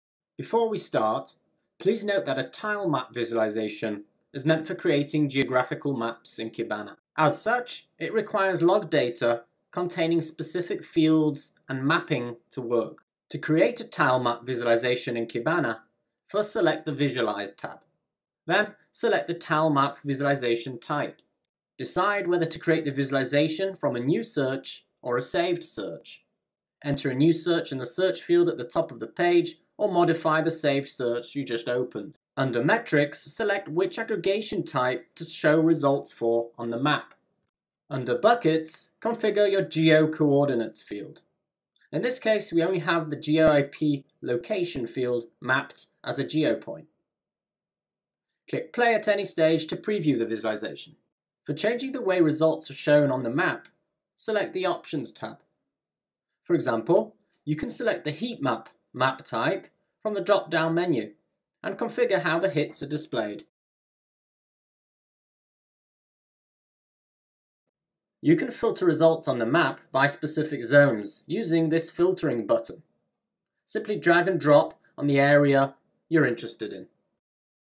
Kibana Visualization How To's - Tile Map
Learn how to create a TileMap visualization in Kibana
This tutorial is one in a series, describing how to work with the different visualization types in Kibana.